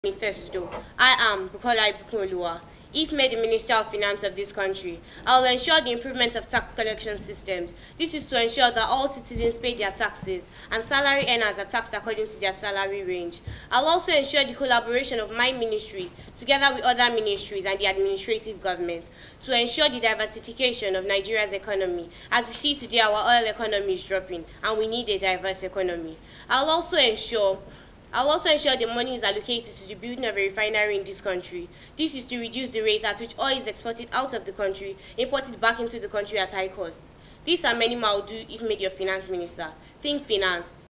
young ministers competition season 2
what a young Nigerian teenager taught about being a minister